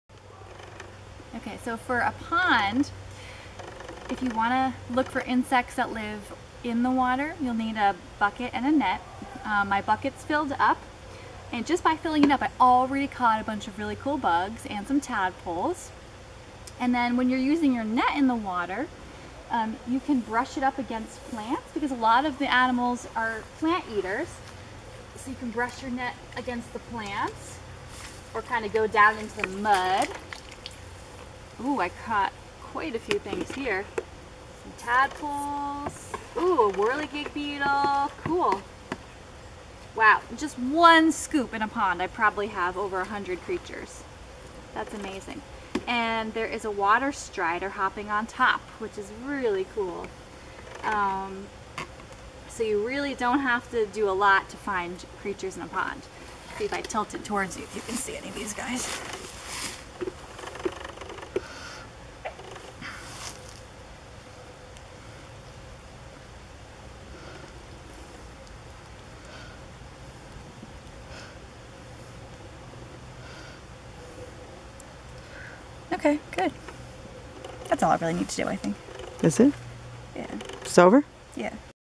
Ms. M's Summer of Bugs: Finding Bugs in a Pond
Join Willow science teacher Ms. M for her Summer of Bugs nature challenge! In this video, learn how to explore your local pond for bugs. Visit willowschool.org to find out more about Ms. M's Summer of Bugs.